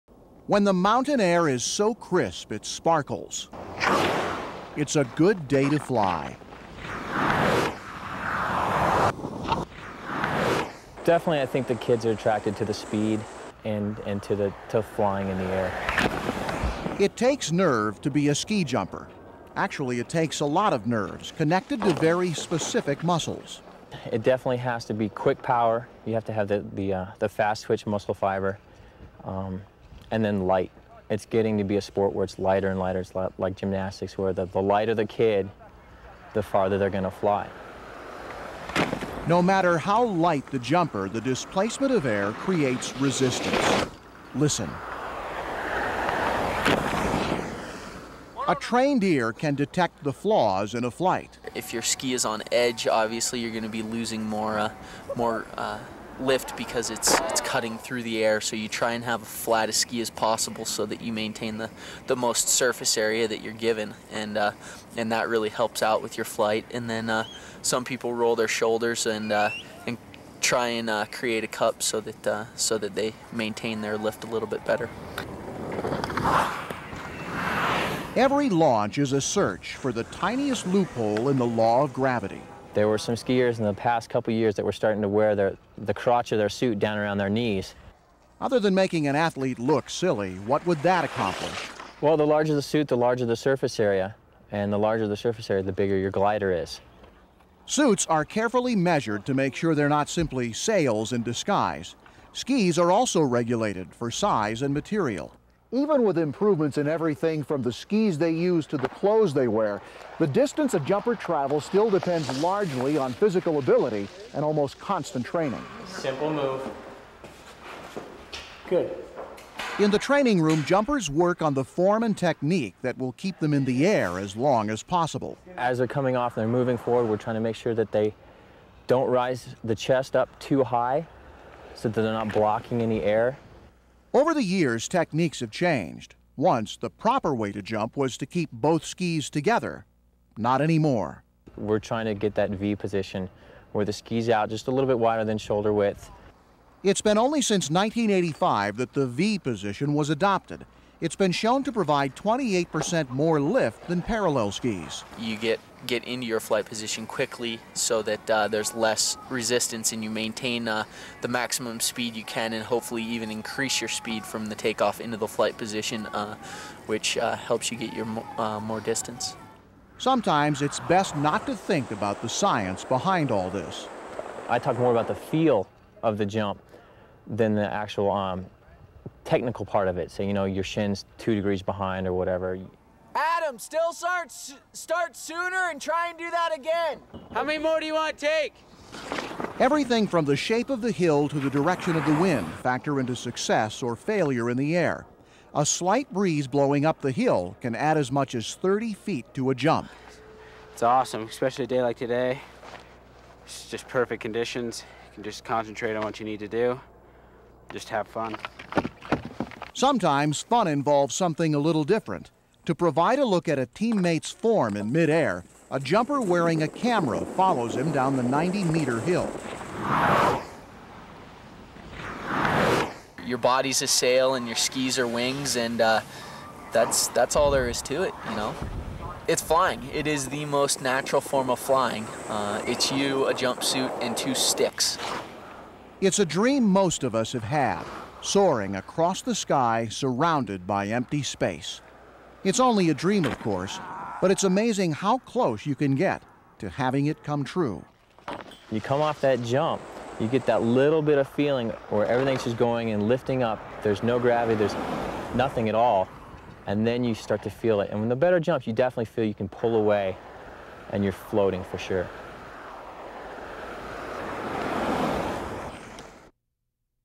Nordic Ski Jumping Steamboat Springs, Colorado.
Training on historic Howelsen Hill. Many Olympic champions were and continue to be built on this small town hill were future champions train every day.  Steamboat is home to more Olympians than any place on earth.